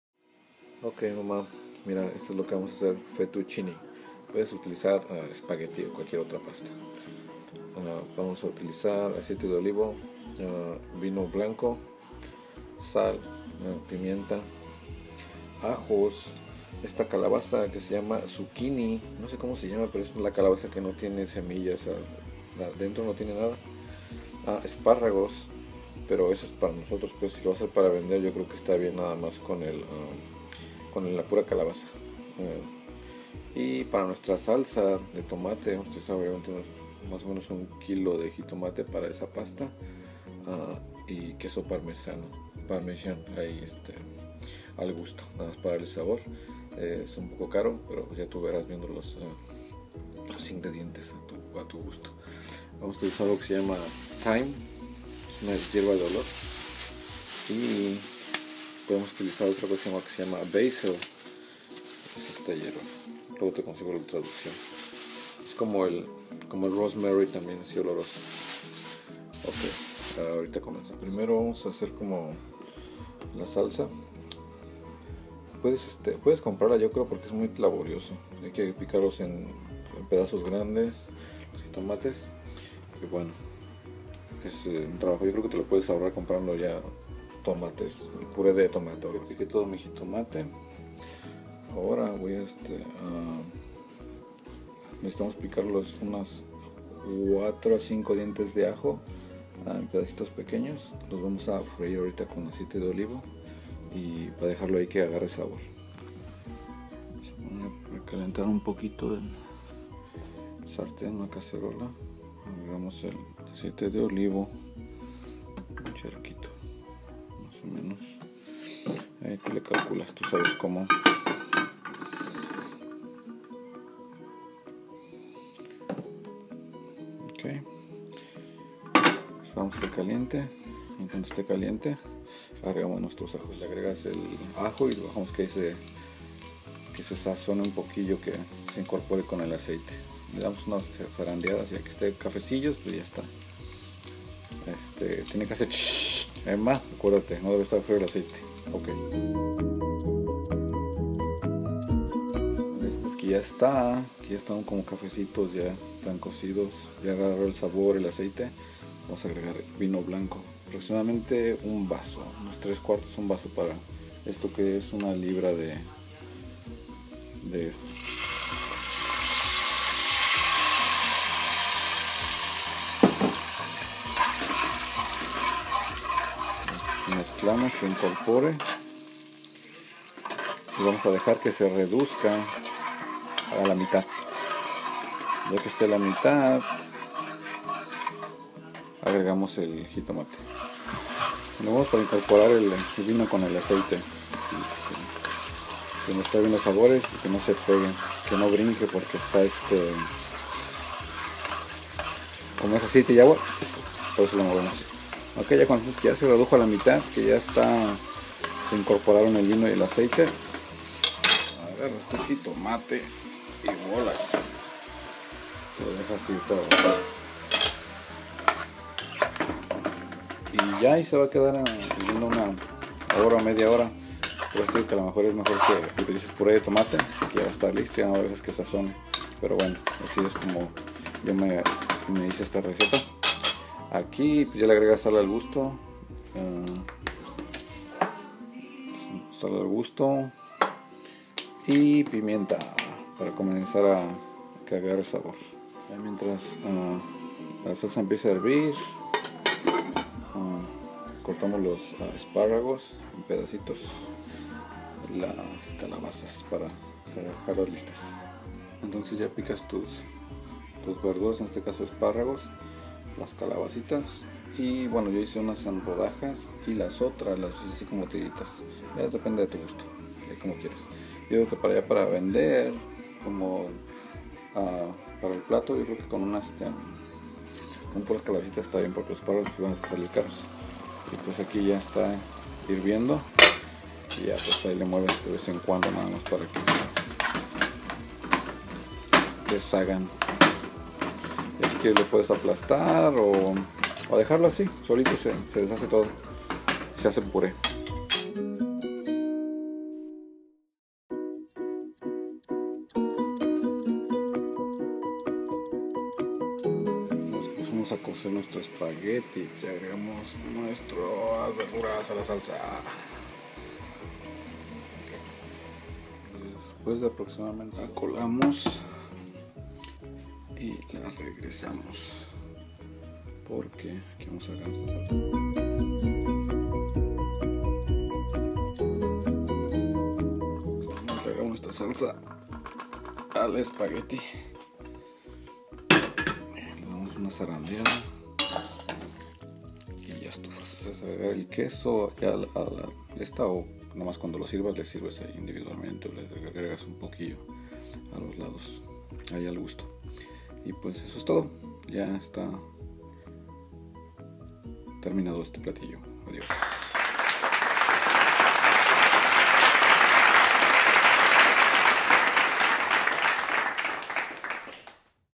Fetuccinni
Como hacer pasta de una receta vista en TV